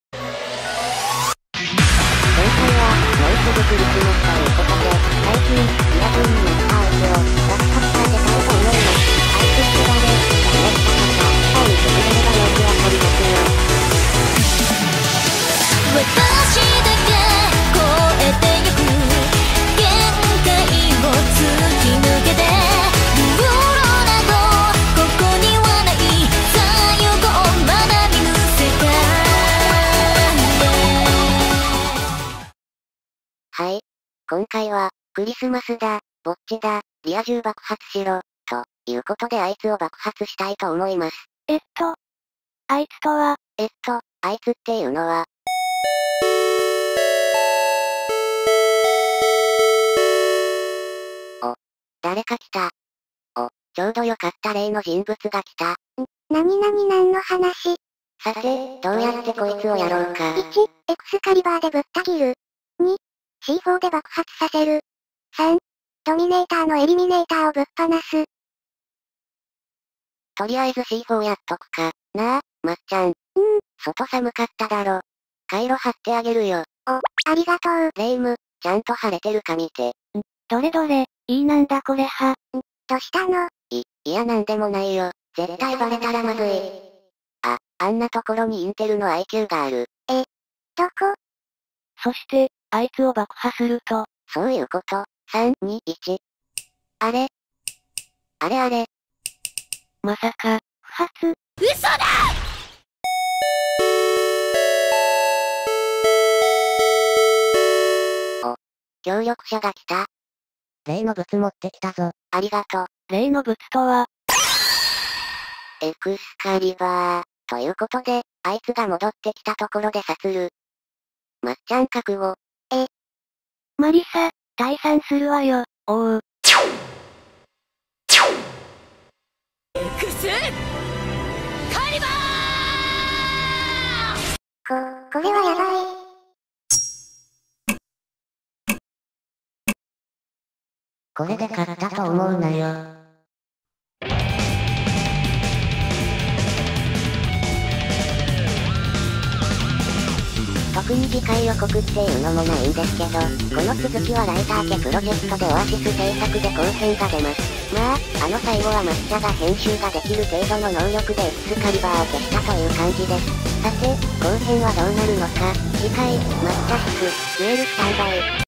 クリスマスだ！ぼっちだ！リア充爆発しろ！【前編】
今回リア充のあいつを爆破します後編は
ライター家Projectででます！

自分が友達だと思っていても相手はそう
思ってないかもしれないよ